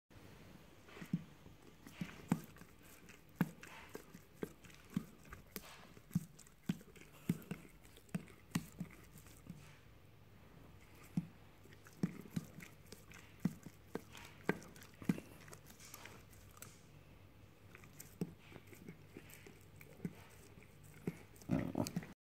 Jerry the Great Dane enjoying his first ice cream
Jerrys first ice cream